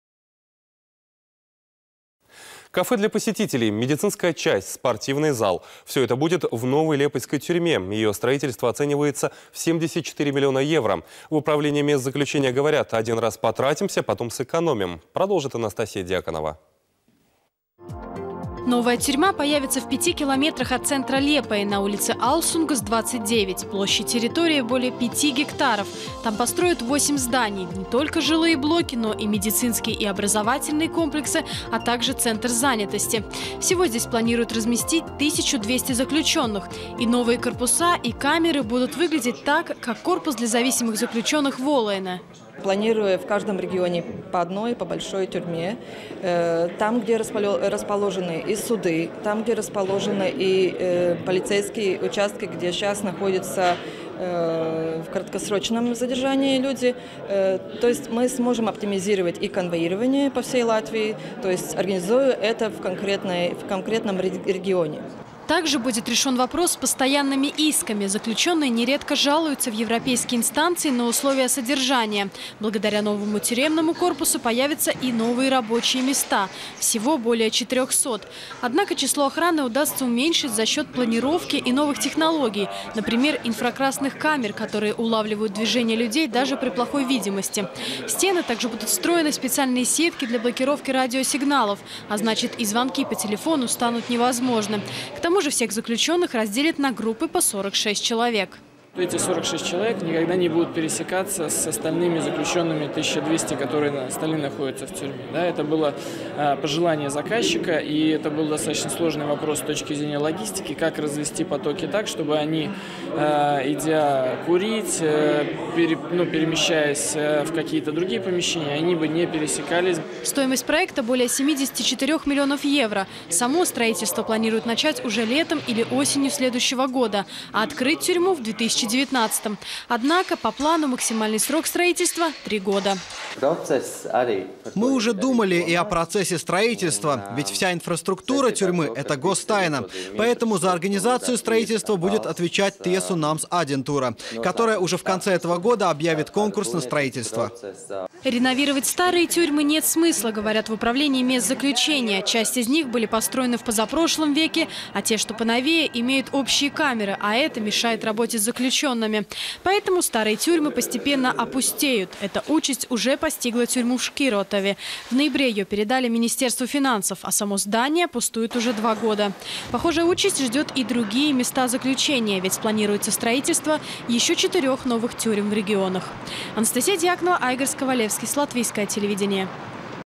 В Лиепае появится тюрьма со всеми удобствами
Кафе для посетителей, медицинская часть, спортивный зал. Всё это будет в новой Лепайской тюрьме. Её строительство оценивается в 74 миллиона евро. В управлении мест заключения говорят — один раз потратимся, потом сэкономим. Продолжит Анастасия Дьяконова.